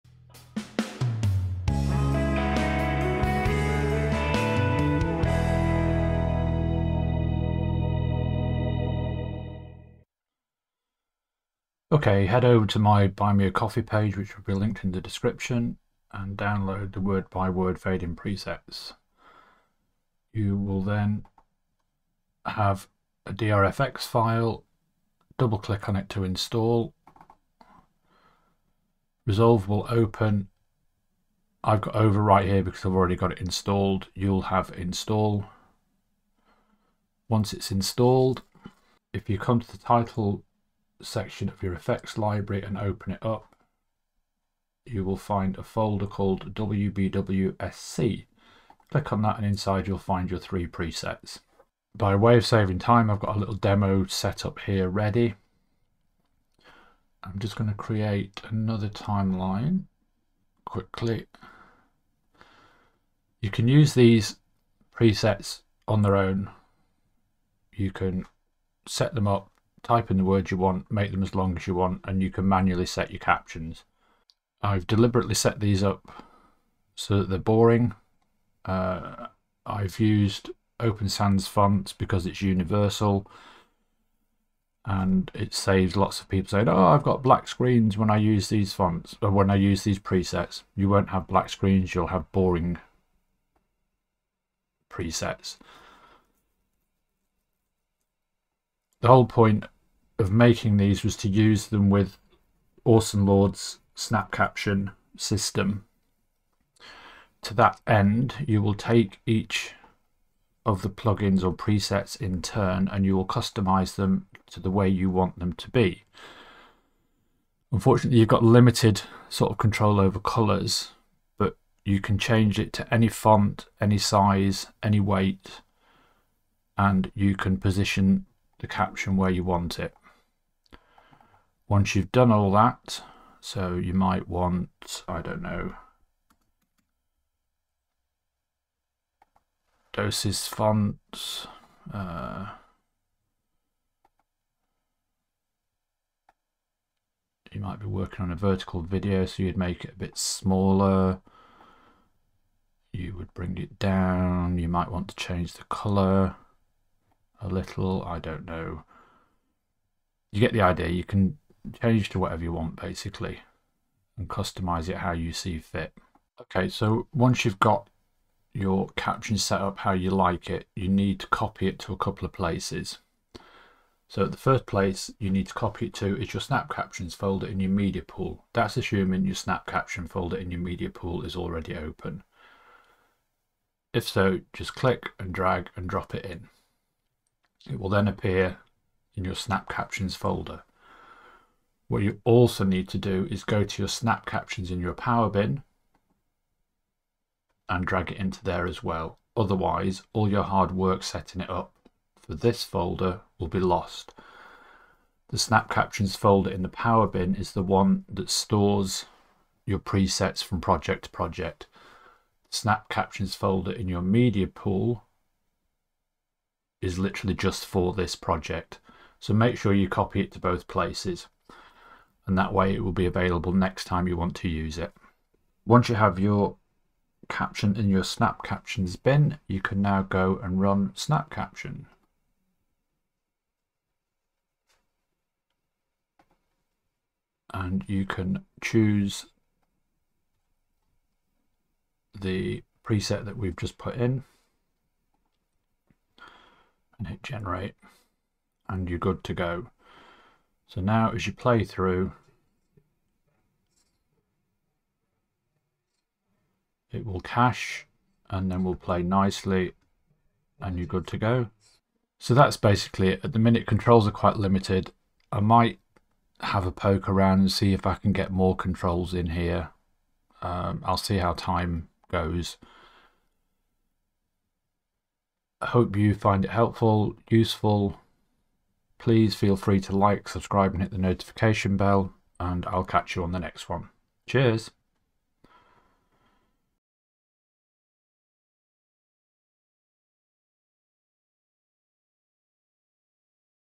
Word by Word Fade In Plus - DaVinci Resolve
When used with @TheOrsonLord's  Snap Caption plugin they make a powerful pairing.
Jake Wipp’s DaVinci Resolve Discord